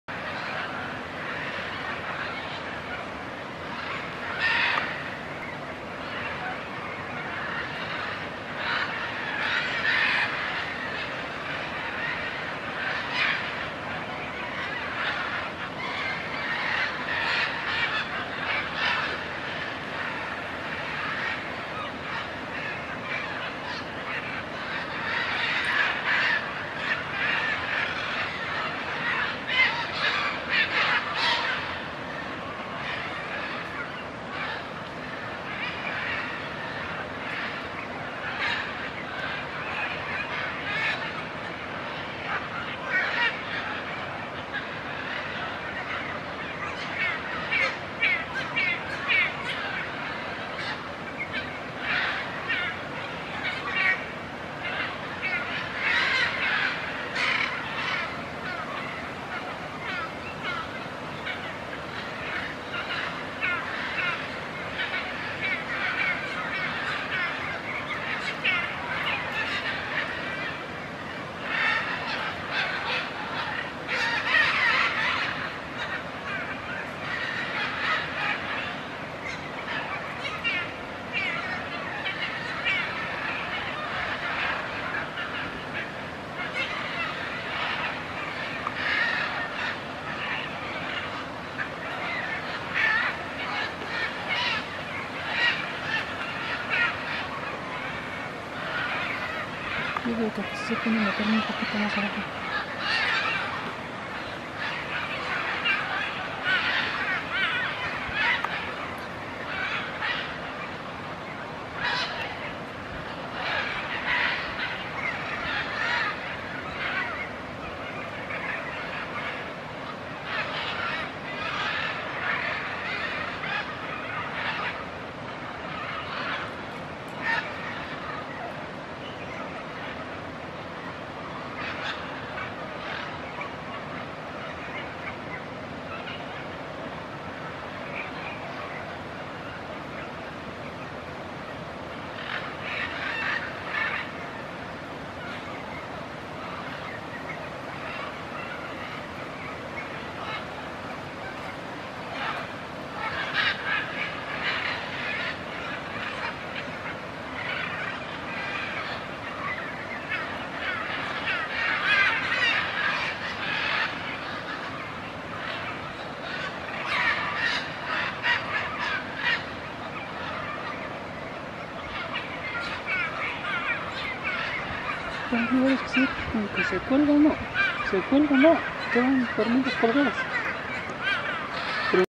Military Macaw behavior in Suaz de Guadalupe, QRO, Mexico
The monitoras who protect and take data on this population shares some interesting behavior during migration time.